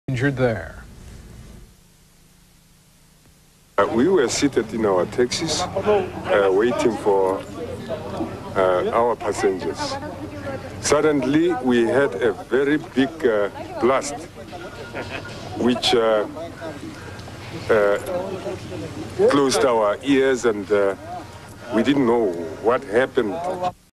1994 Headline NEWS South Africa Randfontein bomb
Pre-election bomb blast 25 April 1994